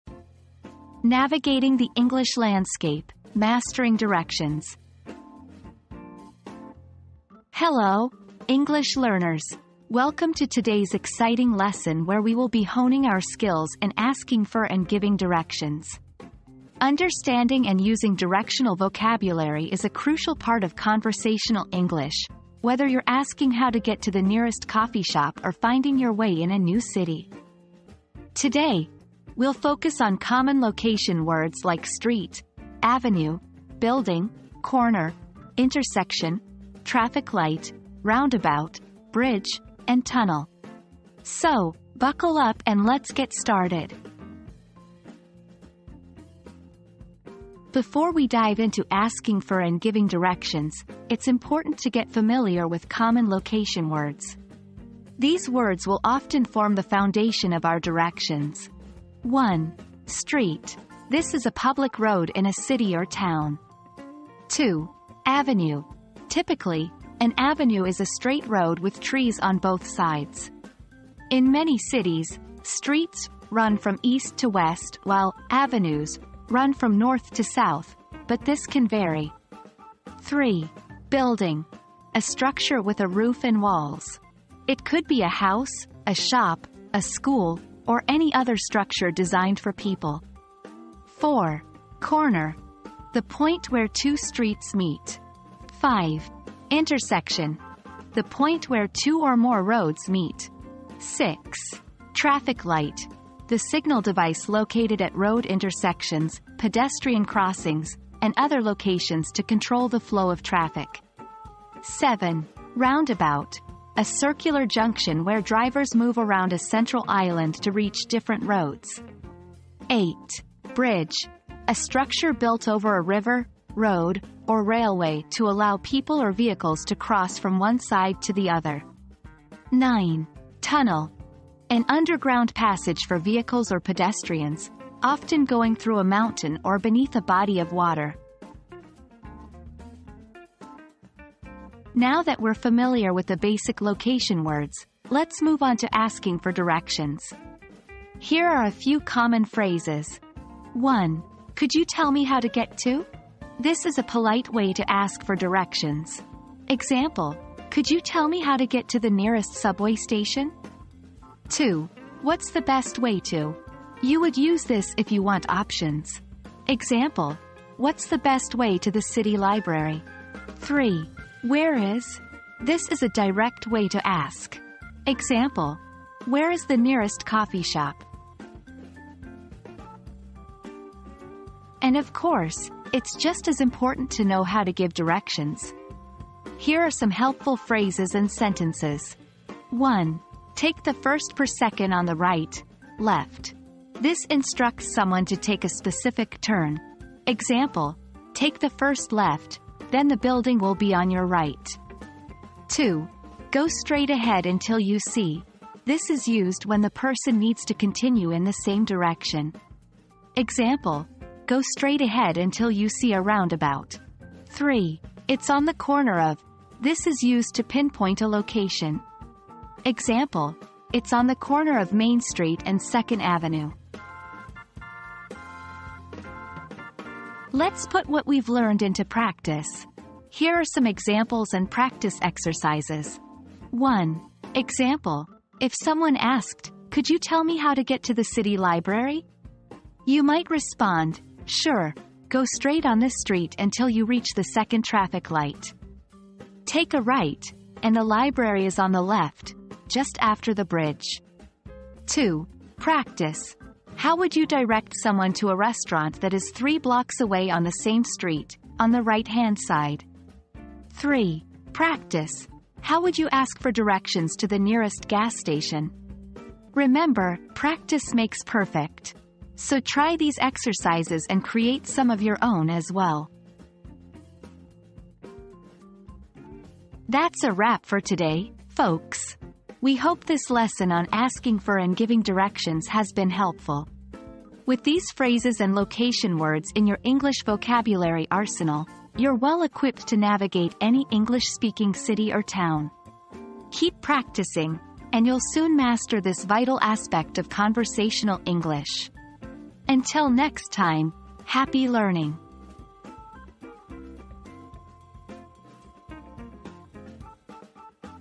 Navigating the English Landscape: Mastering Directions!
Mastering Directions: Navigating the English Landscape • Learn how to confidently ask for and give directions in English with this engaging lesson! Discover common location words and essential vocabulary for navigating the English landscape. Join us as we explore streets, avenues, buildings, corners, intersections, traffic lights, roundabouts, bridges, and tunnels. Get ready to enhance your conversational skills and navigate any city like a pro!

00:00 • Introduction - Navigating the English Landscape: Mastering Directions!
00:47 • Understanding Common Location Words
02:21 • Asking for Directions
03:01 • Giving Directions
03:46 • Examples and Practice Exercises